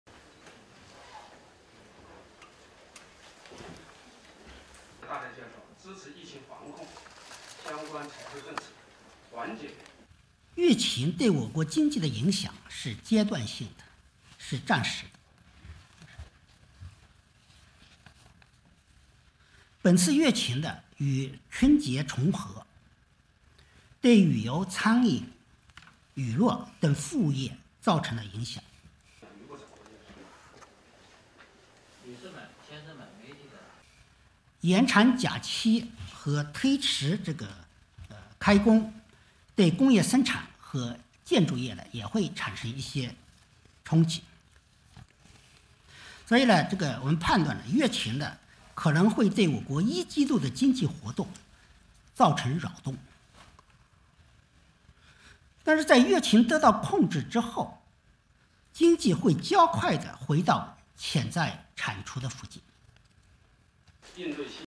China central bank says coronavirus impact on economy will be temporary
China's central bank said on Friday (February 7) the impact from the coronavirus outbreak will be temporary and the country's economy remains resilient.

Vice governor of Chinese central bank Pan Gongsheng said at a news conference that the economy will "return quickly to potential output" after the epidemic is brought under control.

The central bank, in a statement issued ahead of the press briefing, said China is fully confident and capable of coping with any possible impact from the outbreak, which has killed more than 600 people and infected tens of thousands.

The death toll from the coronavirus outbreak in mainland China reached 636 on Friday with 73 more deaths recorded from the previous day and 3,143 new confirmed infections, bringing the total to 31,161 cases, the National Health Commission said. China has vowed repeatedly to be open and transparent in dealing with the situation.